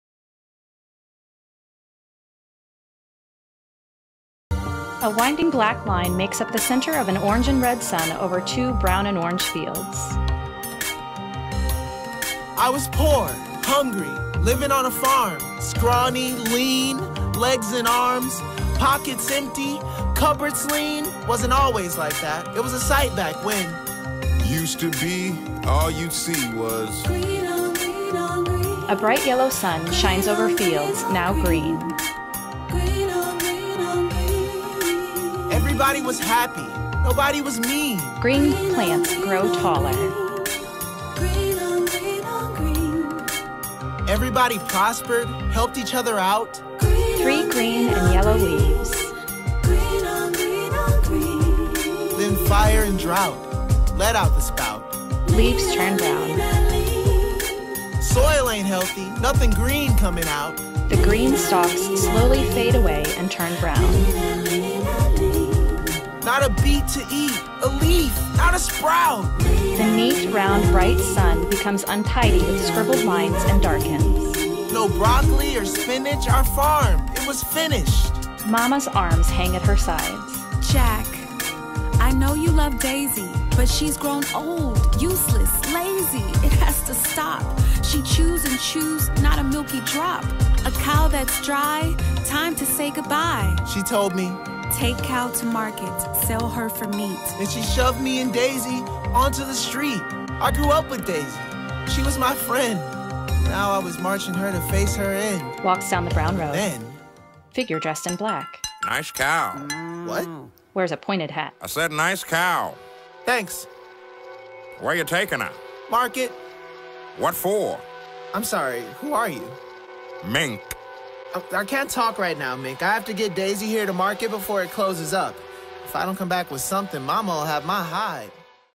Preview of Jacked! with Audio Description
What is audio description? Enjoy this sample of audio description by Magan Harms of MindsEye.

Jacked!
By Idris Goodwin
Directed by Jamie McKittrick
Metro Theater Company 2021

Inspired by the intelligence and emotional wisdom of young people, we create professional theater, foster inclusive community, and nurture meaningful learning through the arts.